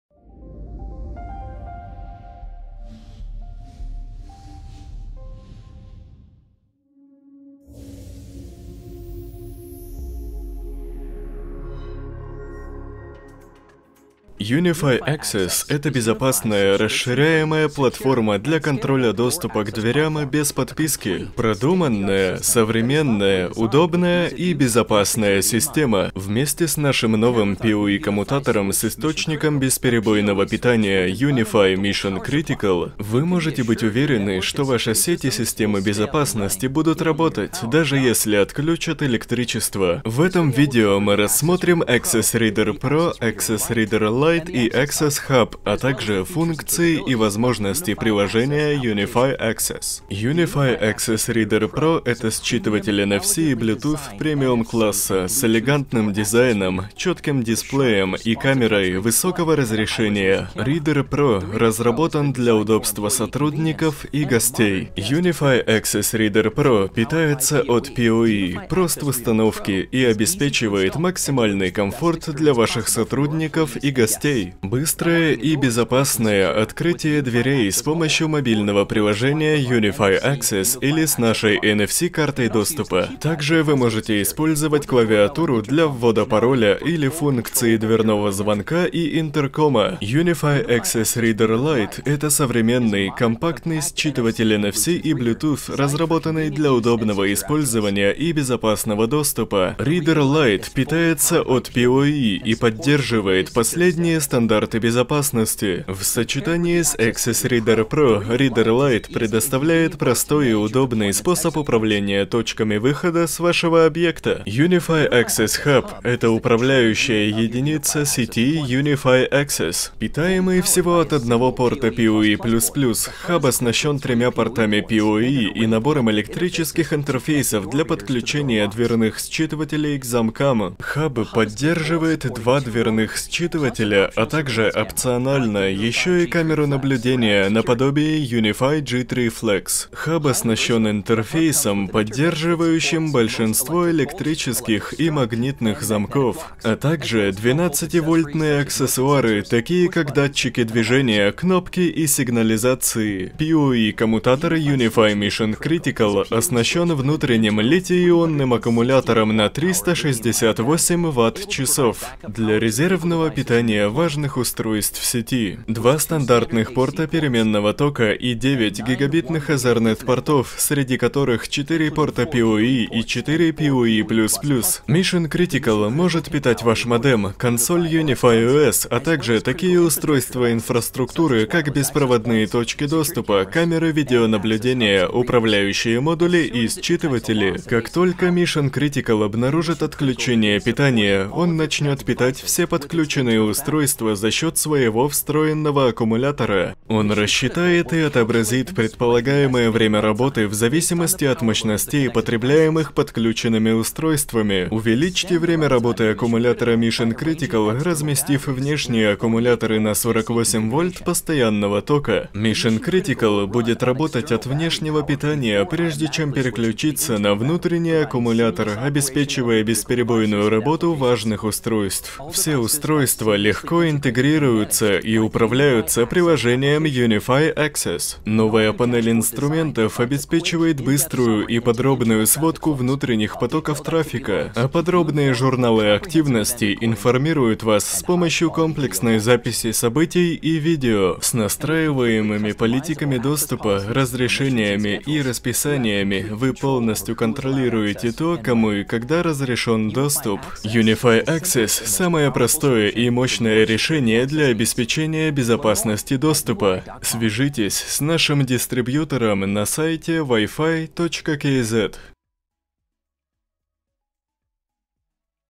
Ubiquiti UniFi Access
Стартовый комплект Ubiquiti UniFi Access - это полноценный набор устройств для установки системы управления доступом UniFi Access. Комплект включает в себя блок управления, два типа бесконтактных считывателей и 20 карт доступа, все необходимые компоненты, предназначенные для установки в домах, офисах или на предприятиях.

Стартовый комплект Ubiquiti UniFi Access совместим с большинством электромеханических и электромагнитных замков и защёлок для запирания дверей. Это позволяет организовать систему доступа практически на любом объекте с минимальными затратами. Централизованная система контроля обеспечивает простое и удобное управление пользователями, гостями, NFC картами, настройку расписания, определение правил доступа, получение отчётов и статистики.

Электропитание подаётся от концентратора на считыватели по стандарту PoE 802.3af, делая подключение максимально простым. Система поддерживает современные стандарты безопасности NFC, включая MIFARE DESFire с шифрованием AES-128.